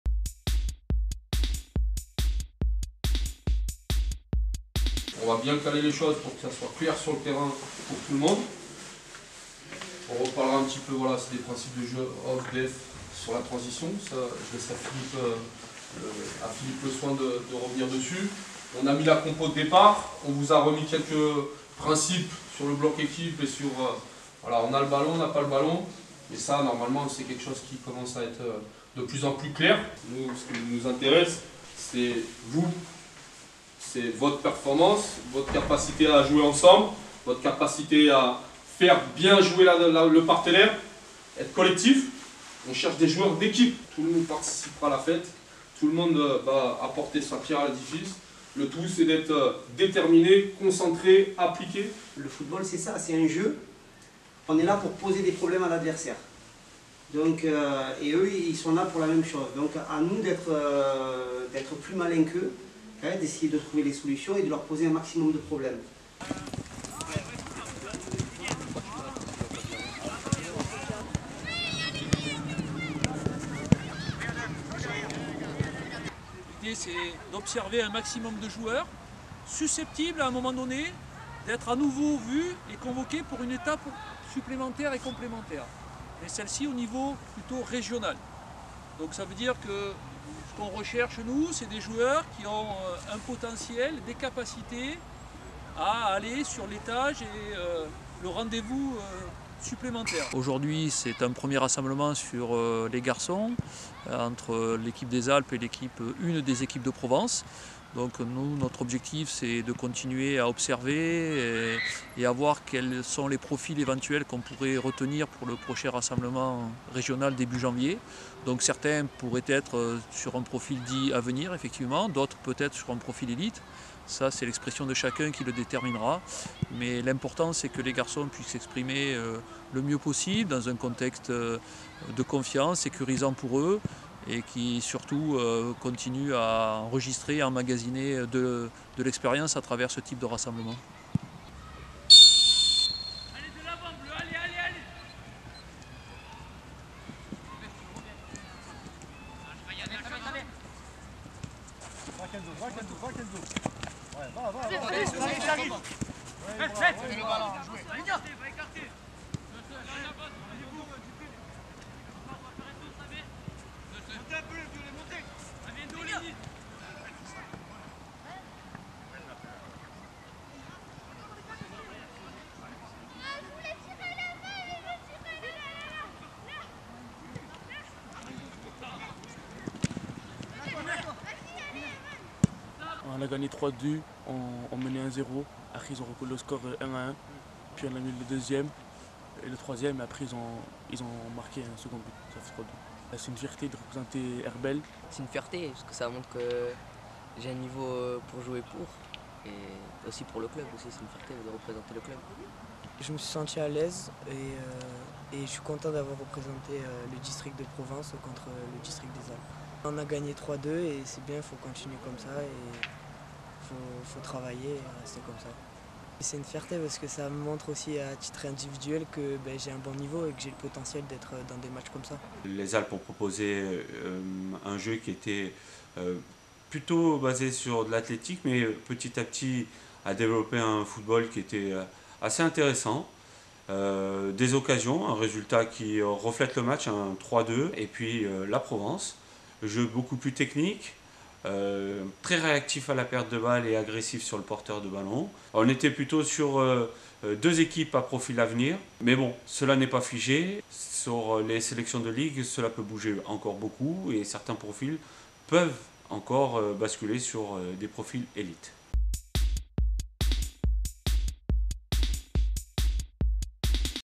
Interdistricts U15G Provence 3 - 2 le 20/11/19 à Venelles
Retour en images sur l'interdistricts U15 Garçons du 20 novembre 2019 à Venelles, entre le District de Provence et celui des Alpes. Victoire 3 buts à 2 de notre sélection provençale.